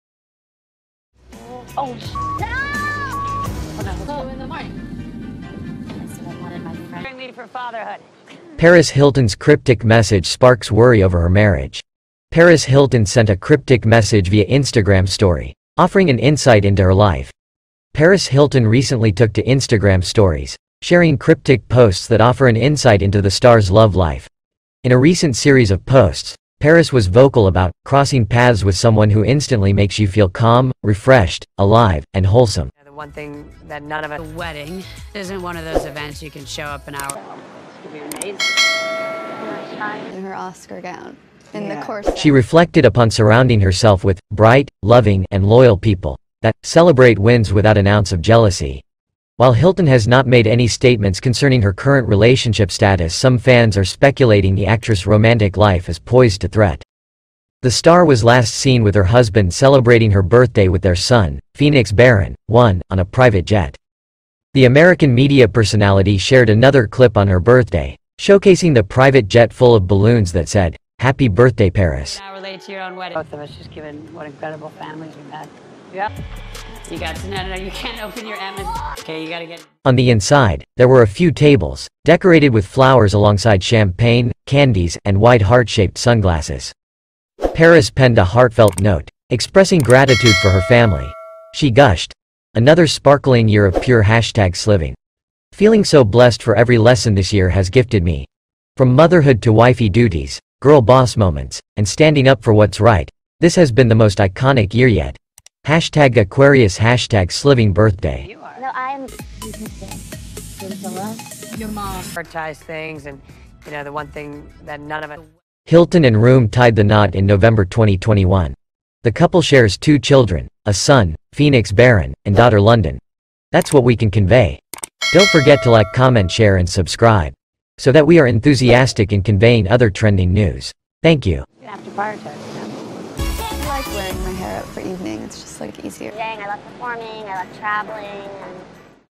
Paris Hilton Cryptic Message Sparks Worry Over Her Marriage Offering An Insight Into Her Life
Paris Hilton Cryptic Message Sparks Worry Over Her Marriage Offering An Insight Into Her Life paris hilton,paris in love,reality tv